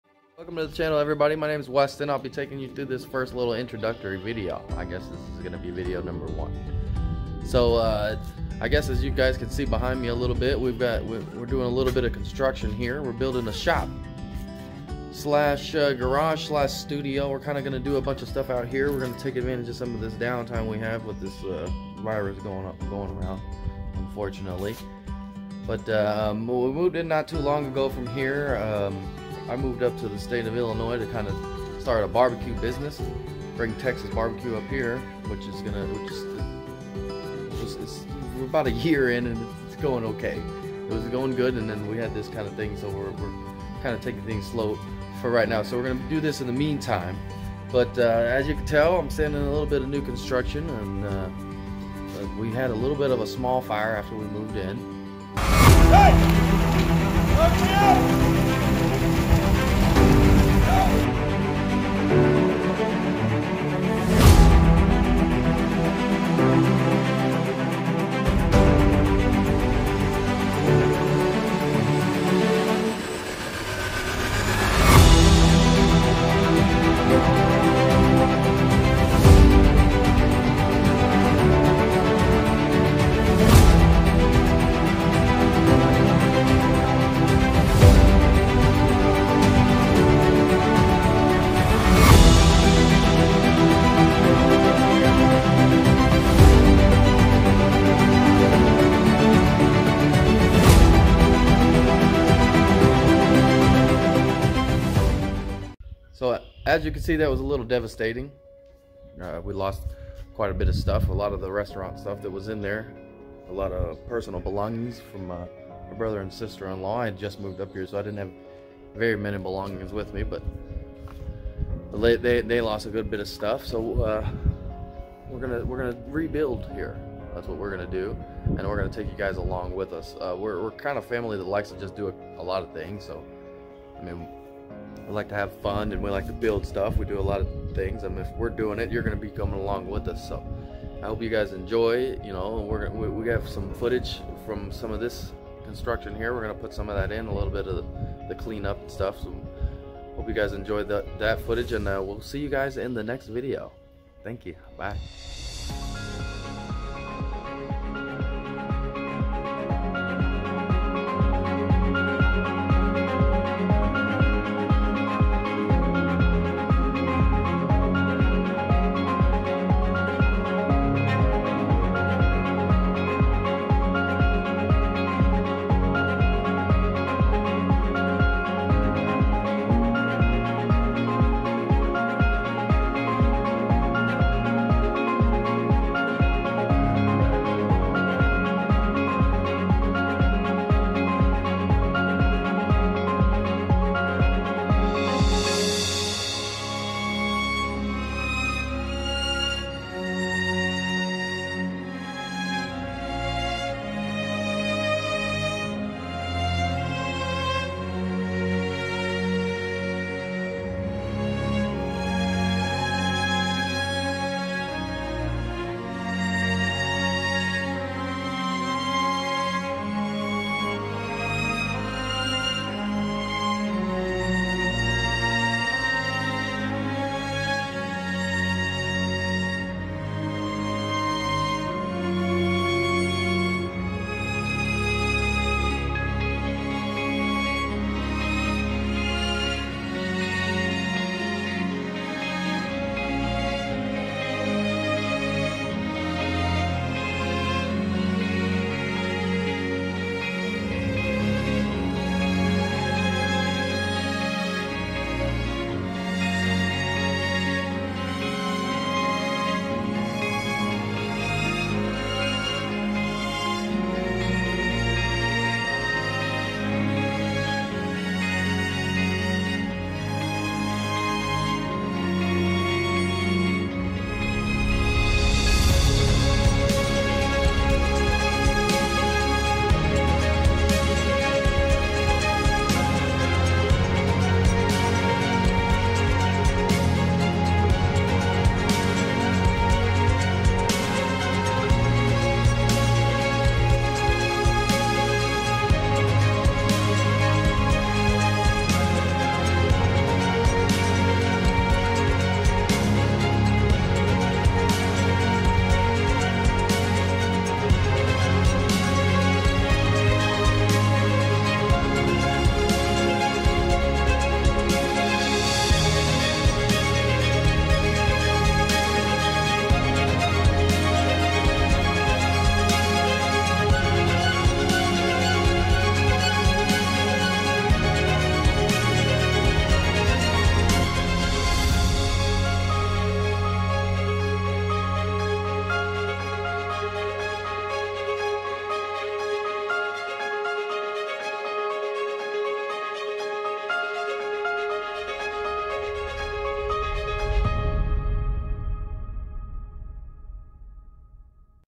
Channel Intro
Welcome to "Gradert Family Makes" Our start-up YouTube Channel!! We are excited to bring you along on our journey and look forward to seeing where this brings us. Come on now, lets go!!